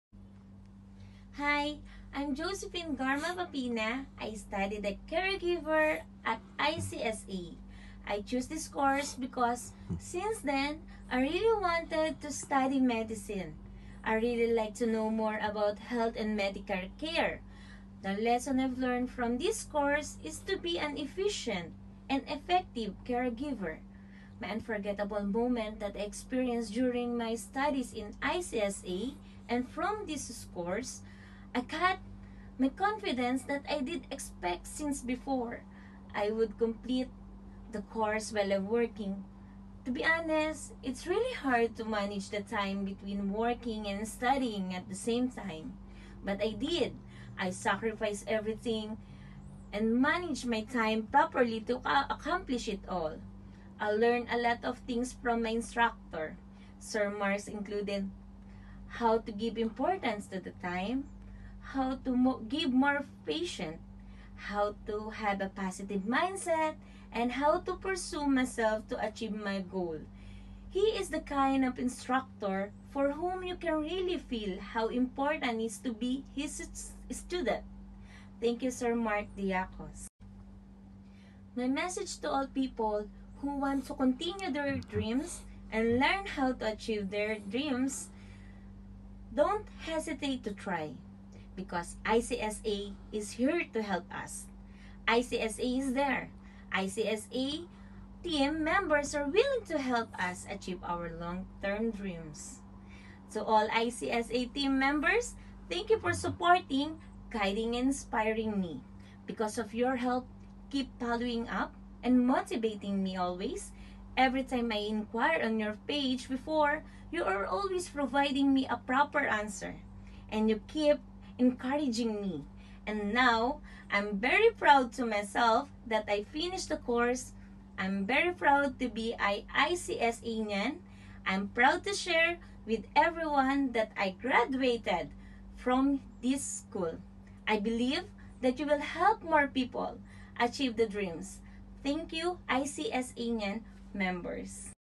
Kwentong ICSA featuring 𝐌𝐬. 𝐉𝐨𝐬𝐞𝐩𝐡𝐢𝐧𝐞 𝐏𝐚𝐩𝐢𝐧𝐚 HSC Student
"I really like to know more about health and medical care. The lesson I learn in this course is to be efficient and effective Caregiver. I'm very proud to myself that I finish the course!"
-𝐌𝐬. 𝐉𝐨𝐬𝐞𝐩𝐡𝐢𝐧𝐞 𝐏𝐚𝐩𝐢𝐧𝐚
𝐃𝐢𝐩𝐥𝐨𝐦𝐚 𝐢𝐧 𝐇𝐞𝐚𝐥𝐭𝐡 𝐚𝐧𝐝 𝐒𝐨𝐜𝐢𝐚𝐥 𝐂𝐚𝐫𝐞 𝐋𝐞𝐯𝐞𝐥 𝟒 (𝐂𝐚𝐫𝐞𝐠𝐢𝐯𝐞𝐫 𝐔𝐊 𝐐𝐮𝐚𝐥𝐢𝐟𝐢𝐜𝐚𝐭𝐢𝐨𝐧𝐬)
Look for Ms. Reshell
𝙑𝙞𝙙𝙚𝙤 𝘾𝙤𝙪𝙧𝙩𝙚𝙨𝙮 𝙤𝙛 𝙈𝙨. 𝙅𝙤𝙨𝙚𝙥𝙝𝙞𝙣𝙚 𝙋𝙖𝙥𝙞𝙣𝙖